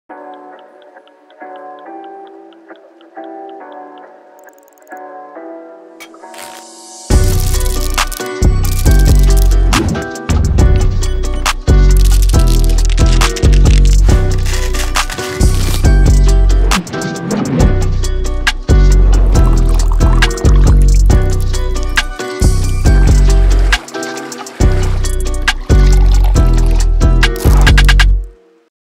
Coffee Epic Cinematic handheld B Roll commercial movie Inspired by Daniel Schiffer
By: Enzio Nguyen - Photographer based in Ho Chi Minh City, Viet Nam
GEAR USED IN THIS VIDEO:
Sony A7III
Tamron 28-75mm f2.8
Godox SL60W
Aputure AL MC